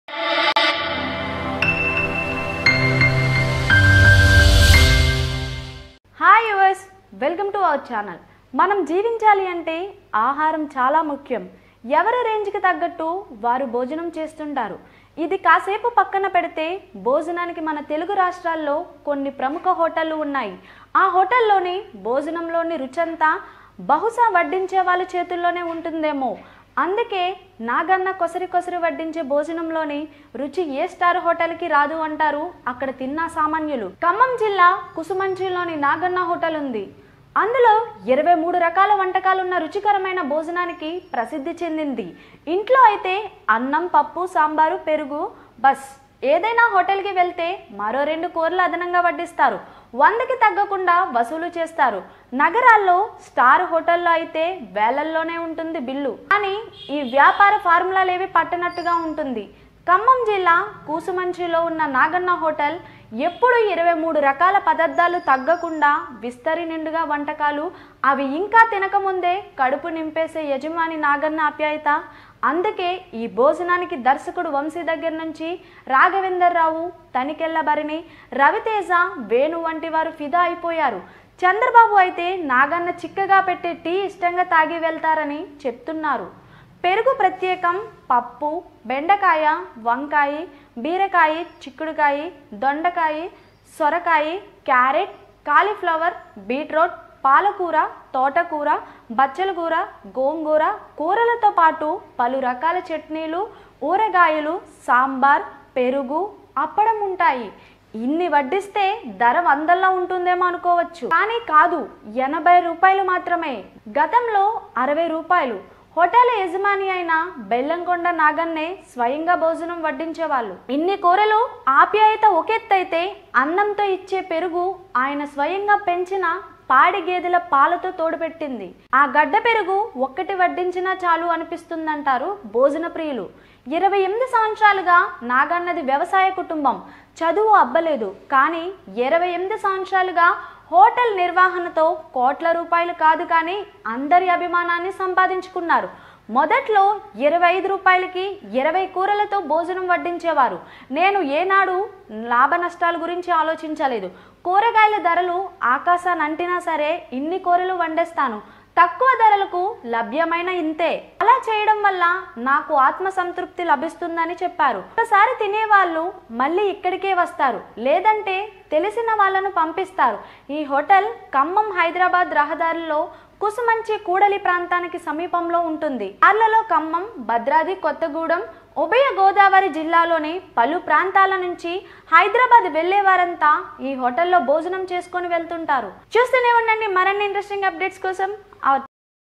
Naganna Hotel In Khammam District | Naganna Mess FamousFor Best South Indian Meals | Tollywood Nagar
Khammam District Kusumanchi Naganna Hotel Most Famous For
26 Varieties Of Best South Indian Meals. Most Famous Persons Like Chandrababu, Chiranjeevi, Raghavendrarao Always Visits the hotel.

Hey guys!